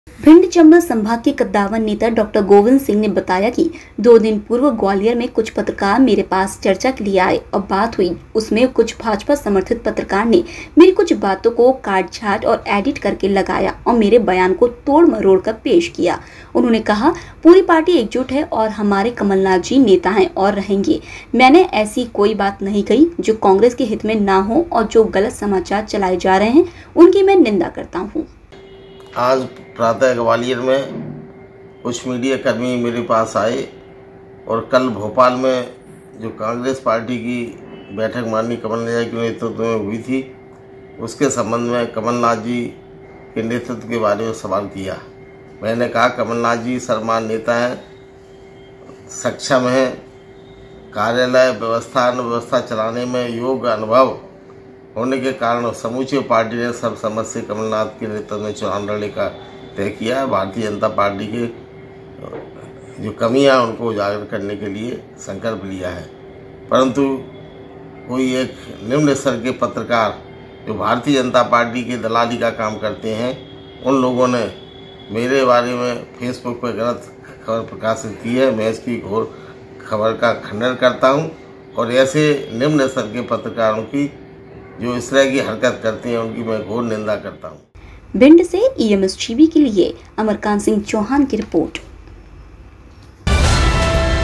झूठी ख़बर पर बिफरे कांग्रेस नेता, बोले कमलनाथ ही हमारे नेता | EMS TV 6-April-22
मंत्री डॉ. गोविंद सिंह बोले कमलनाथ ही हमारे नेता
झूठी ख़बर पर बिफरे मंत्री डॉ. गोविंद सिंह

भिंड चंबल संभाग के कद्दावर नेता और पूर्व मंत्री डॉक्टर गोविंद सिंह ने बताया कि 2 दिन पूर्व ग्वालियर में कुछ पत्रकार मेरे पास चर्चा के लिए आए और बात हुई उसमें कुछ भाजपा समर्थित पत्रकार ने मेरी कुछ बातों को काट छांट और एडिट करके लगाया और मेरे बयान को तोड़ मरोड़ कर पेश किया उन्होंने कहा पूरी पार्टी एकजुट है और हमारे कमलनाथ हमारे नेता है और रहेंगे मैंने ऐसी कोई बात नहीं कही जो कांग्रेस के हित में न हो और जो गलत समाचार चलाए जा रहे हैं उनकी मैं निंदा करता हूं